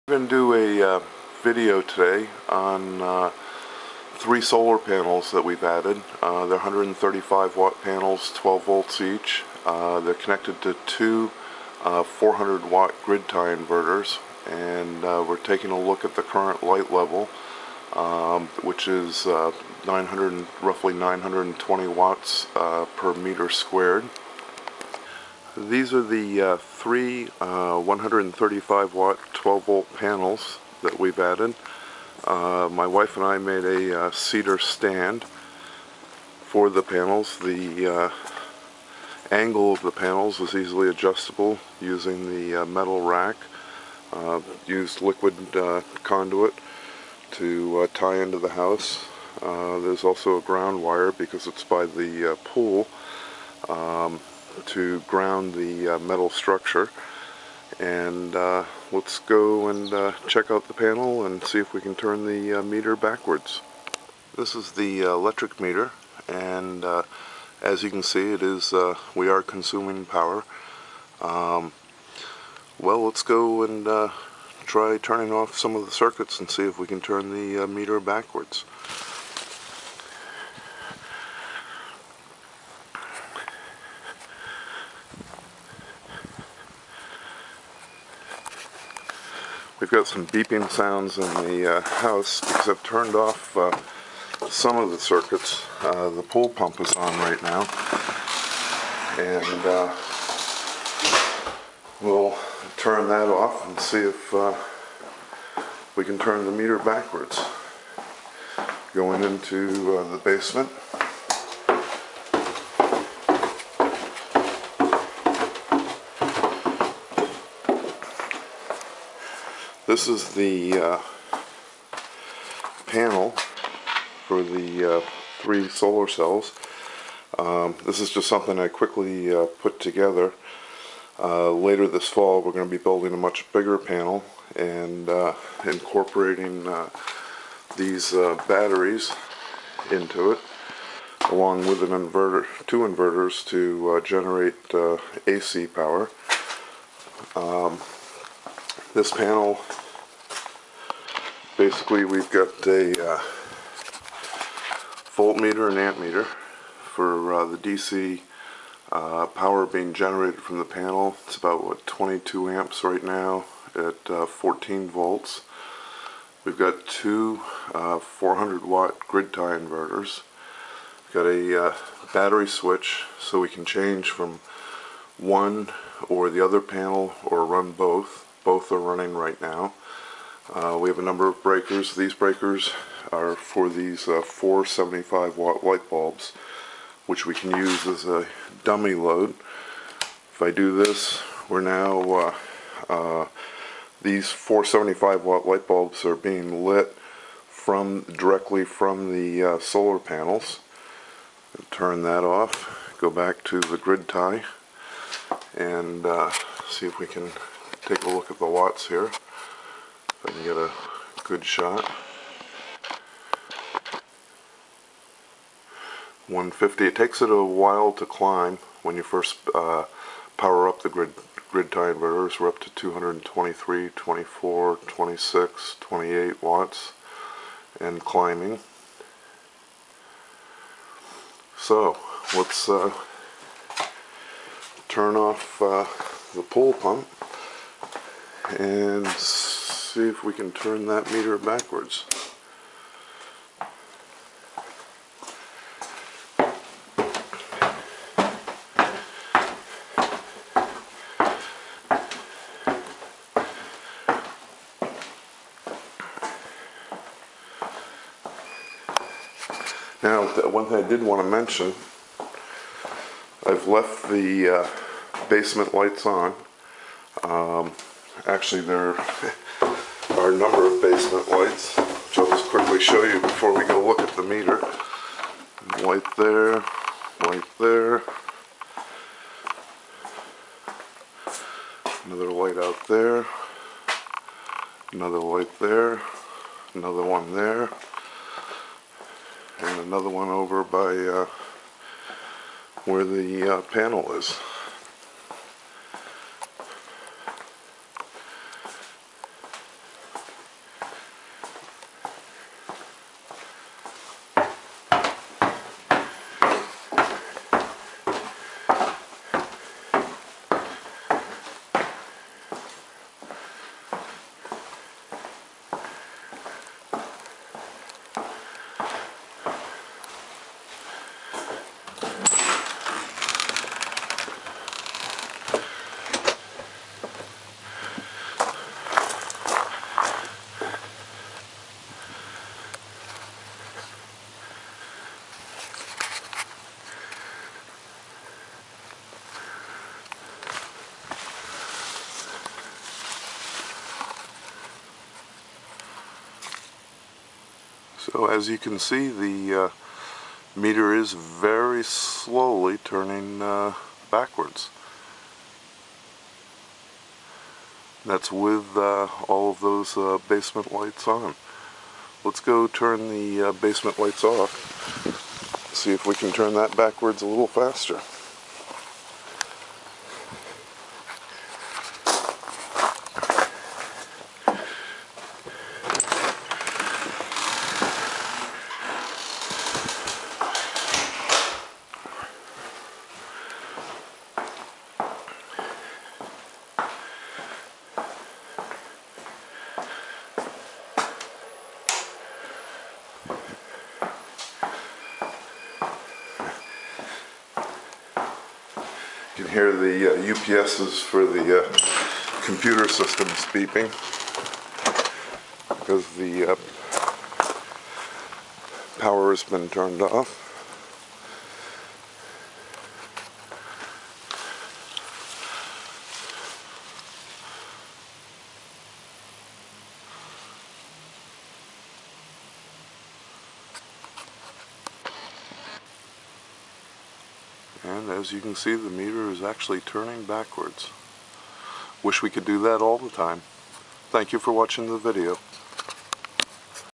Solar System Turning Power Meter Backwards
Three 135 Watt 12 Volt photovoltaic panels and two 400W grid-tie inverters turning AC power meter backwards with lights on.